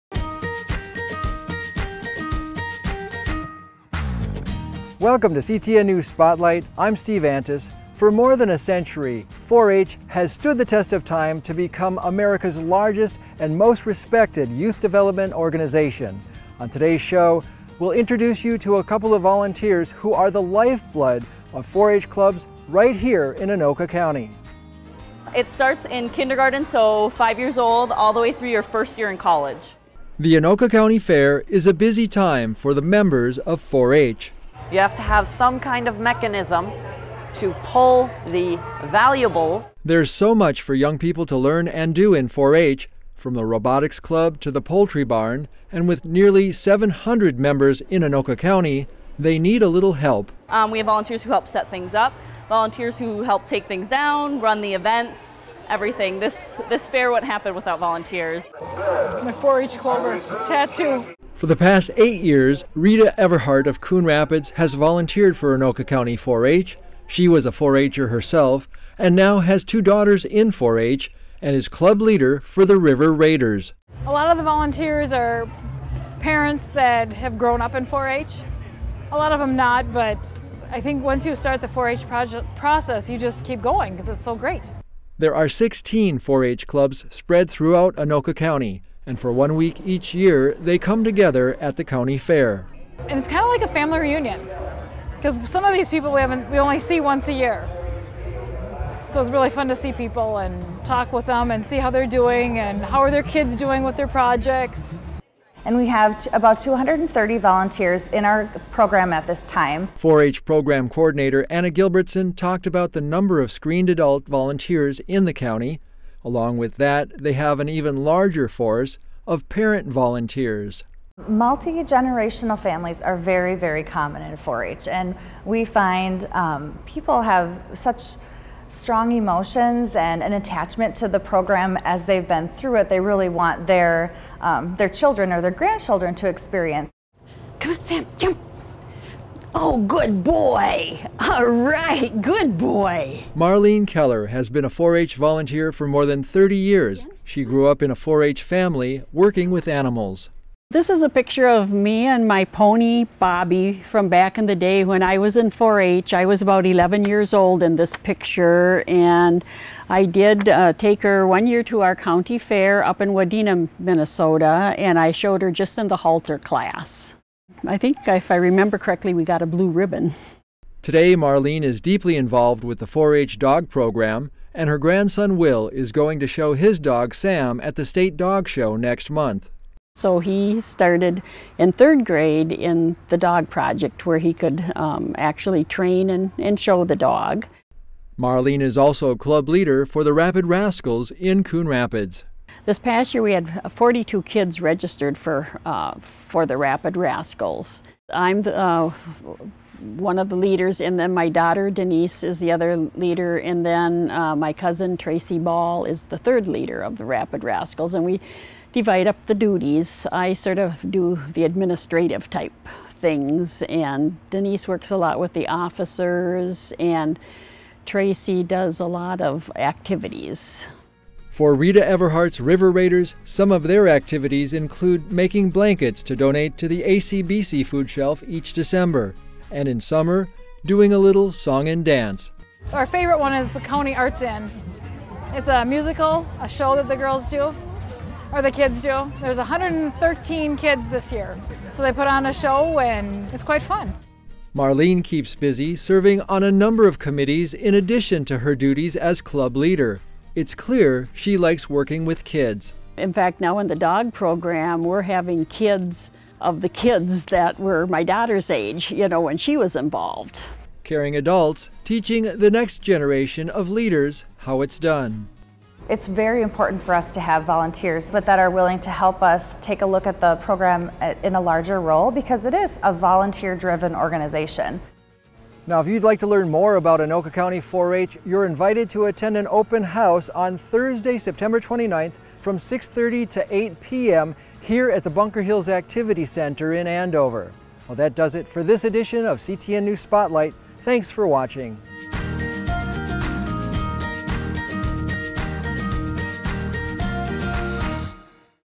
CTN News Spotlight: Anoka County 4-H
This video is about the Anoka County 4-H program.